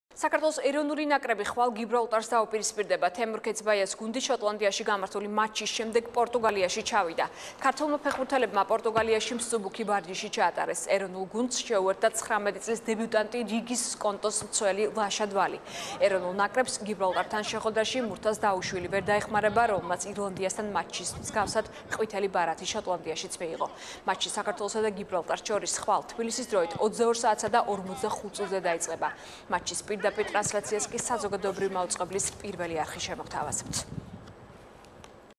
საქართველო გიბრალტართან შეეცდება ქულების მოპოვებას
საქართველოს ეროვნული ნაკრები ხვალ გიბრალტარს დაუპირისპირდება. შოტლანდიაში გამართული მატჩის შემდეგ თემურ ქეცბაიას გუნდი პორტუგალიაში ჩავიდა. ქართველმა ფეხბურთელებმა პორტუგალიაში მსუბუქი ვარჯიში ჩაატარეს. ეროვნულ გუნდს შეუერთდა 19 წლის დებიუტანტი რიგის "სკონტოს" მცველი, ლაშა დვალი. ეროვნულ ნაკრებს გიბრალტართან შეხვედრაში მურთაზ დაუშვილი ვერ დაეხმარება, რომელმაც ირლანდიასთან და შოტლანდიასთან გამართულ მატჩებში ორი ყვითელი ბარათი მიიღო. მატჩი საქართველოსა და გიბრალტარს შორის ხვალ თბილისის დროით 22 საათსა და 45 წუთზე დაიწყება. მატჩის პირდაპირ ტრანსლაციას საზოგადოებრივი მაუწყებლის პირველი არხი შემოგთავაზებთ